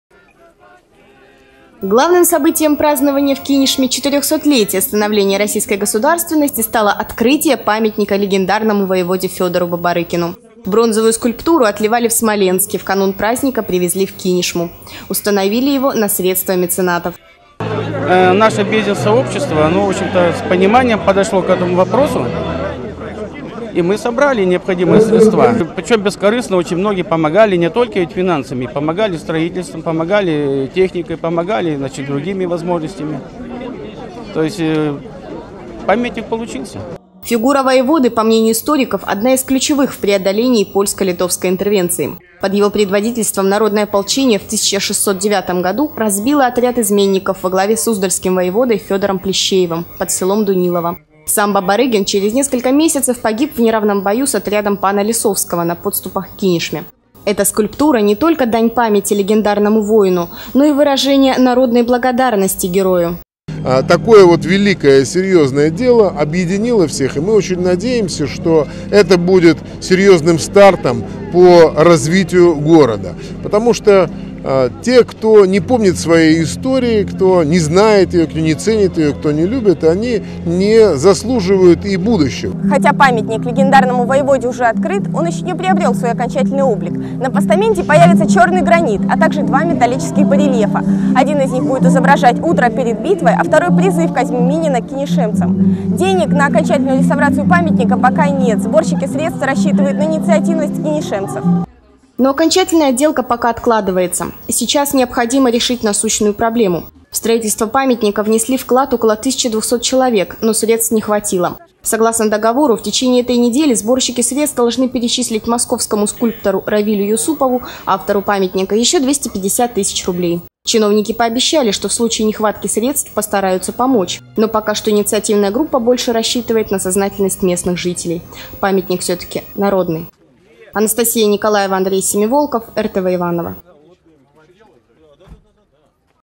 400-летие становления российской государственности
Главным событием празднования в Кинешме 400-летия становления российской государственности стало открытие памятника легендарному воеводе Федору Боборыкину.